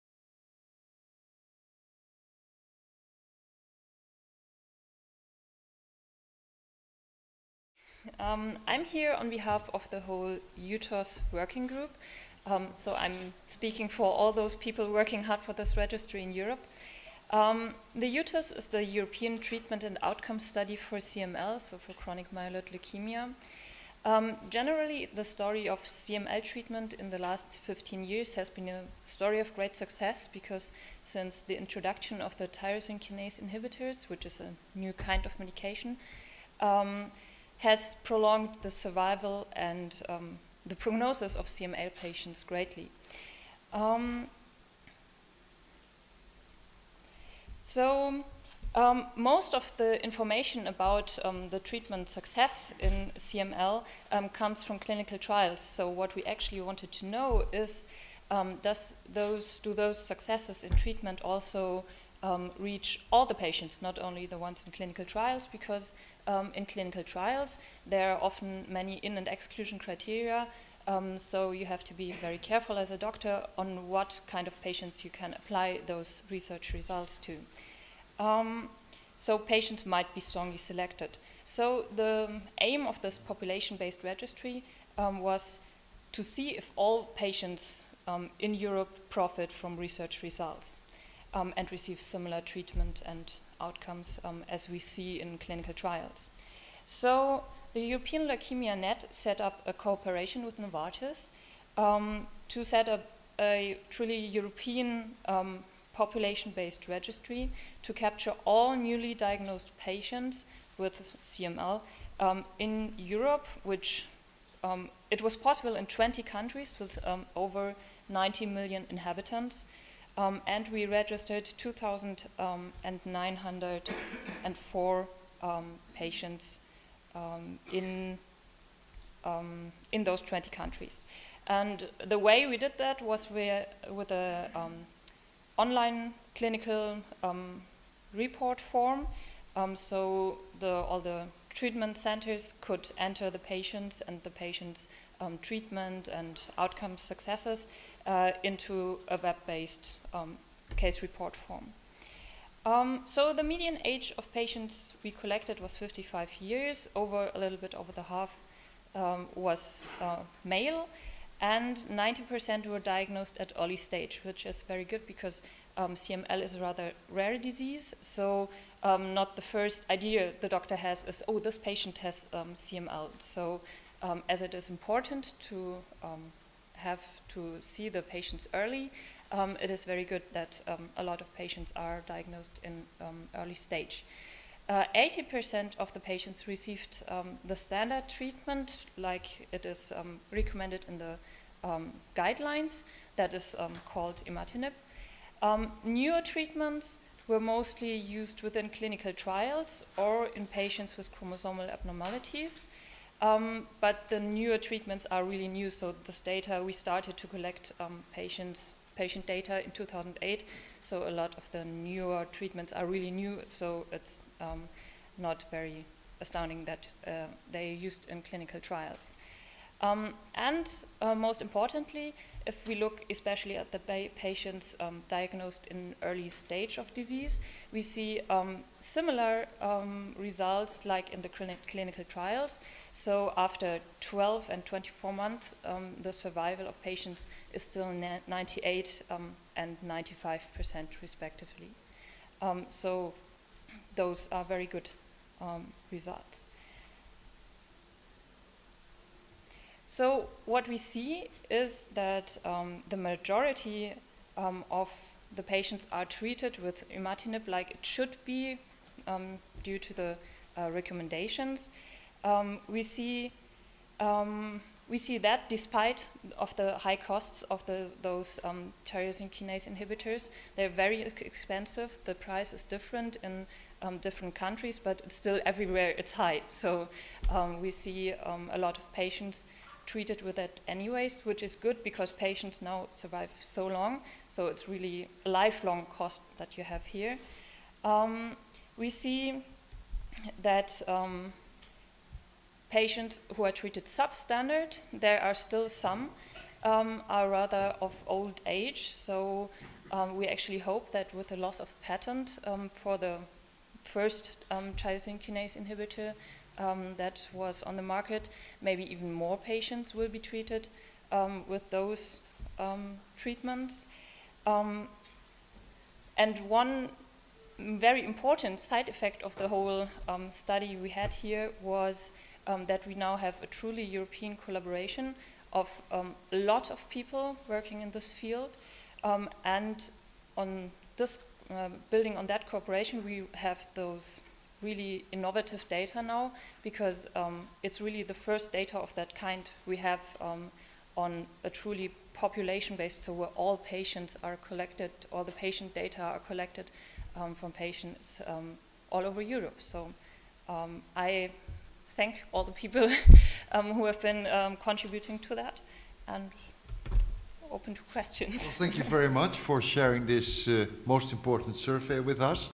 Treatment and outcome of CML patients from the EUTOS population-based registry
The European Treatment Outcome Study (EUTOS) investigated the treatment and treatment outcome of patients with chronic myeloid leukaemia (CML).

Data of 2904 newly diagnosed CML patients in 20 countries or prespecified regions of Europe were recorded in a web-based registry. 94% of patients were diagnosed in early stage of disease and received treatment with imatinib according to the European LeukemiaNet (ELN) recommendations. Newer treatment options were used by patients in clinical trials or with chromosomal abnormalities. Sub-standard treatments were used rarely, but mainly in older patients. Patient survival probabilities were similar to clinical trial results after adjusting to risk profile.

Presented by Fr. Dr. rer. biol. hum. Verena Hoffmann, BSc, MA, Ludwig-Maximilians-Universität München, Germany, at the 20th Congress of the European Hematology Association (EHA)